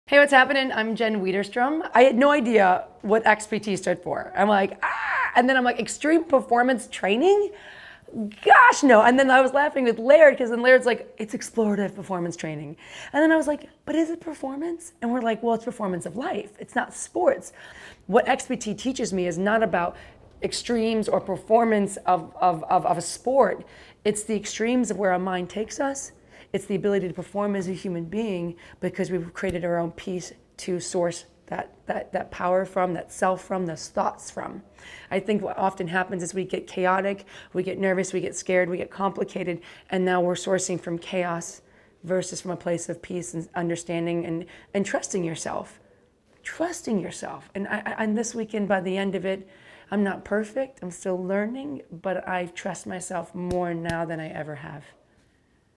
XPT Testimonial Jen Widerstrom
Who doesn’t want to trust themselves more? Better trust in herself is one of the biggest takeaways fitness guru, Jen Widerstrom left the XPTExperience in Kauai with after spending three days soaking up the knowledge shared by Laird Hamilton, Gabby Reece, PJ Nestler and the rest of the XPT Family.

Many people have a false perception of the words Extreme Performance Training and here Jen admits to the same, but listen to her words about her realization of what the program is all about and how being guided through the BreatheMoveRecover lifestyle helped her understand herself better and how to perform better, not in a specific sport, but in all facets of life.

Jen Widerstrom's powerful testimonial of the XPTExperience is not uncommon as many people leave feeling like they are set up with the tools for a successful, more efficient and impactful lifestyle. If you would like to experience this first hand, checkout our upcoming lineup of XPT Experiences and sign-up for one of these three-day, hands-on, fitness lifestyle retreats